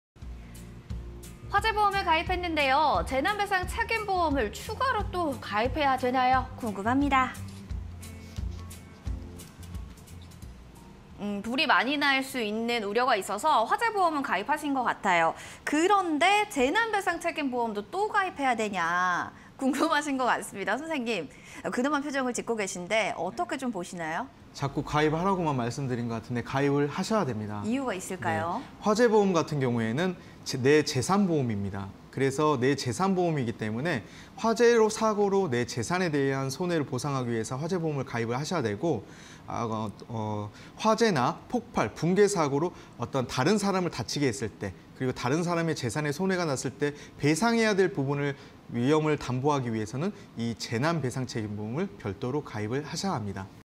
화재보험 가입 경우, 재난배상책임보험을 추가로 가입해야 할까?_보험 Q&A (20210910)
화재보험 가입 경우, 재난배상책임보험을 추가로 가입해야 할까?